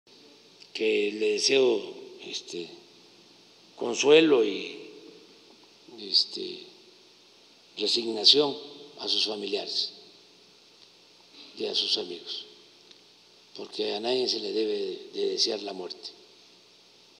La Mañanera EnResumen | AMLO lamenta la muerte de Carlos Romero Deschamps
El presidente López Obrador se refirió a la muerte del exdirigente del Sindicato de Trabajadores Petroleros de la República Mexicana, Carlos Romero Deschamps.

“Le deseo consuelo y resignación a sus amigos y familiares, porque a nadie se les debe desear la muerte”, afirmó durante su conferencia mañanera de hoy, viernes 20 de octubre de 2023.